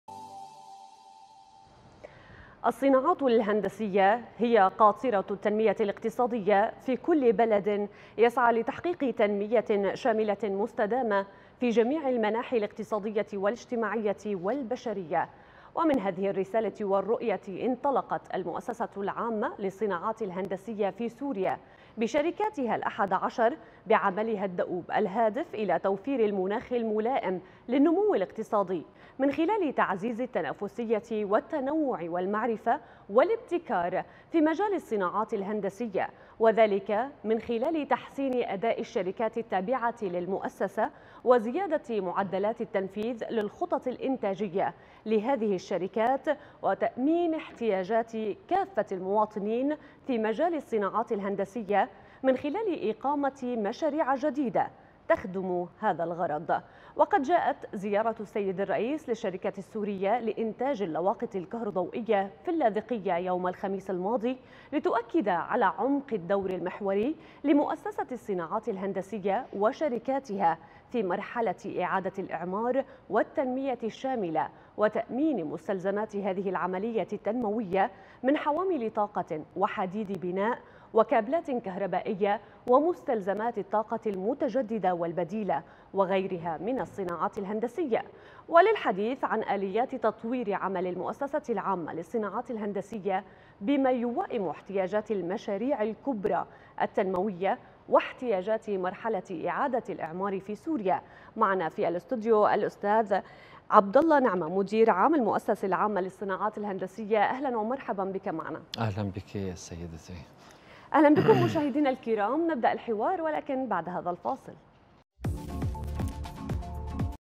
المؤسسة العامة للصناعات الهندسية بشركاتها 11 تسعى بعملها الدؤوب لتوفير مناخ ملائم للنمو الاقتصادي
الصناعات الهندسية هي قاطرة التنمية الاقتصادية في كل بلد يسعى لتحقيق تنمية شاملة مستدامة في جميع المناحي الاقتصادية والاجتماعية والبشرية ومن هذه الرسالة والرؤية انطلقت المؤسسة العامة للصناعات الهندسية في سورية بشركاتها الأحد عشر بعملها الدؤوب الهادف إلى توفير المناخ الملائم للنمو الاقتصادي من خلال تغزيز التنافسية والتنوع والمعرفة والابتكار في مجال الصناعات الهندسية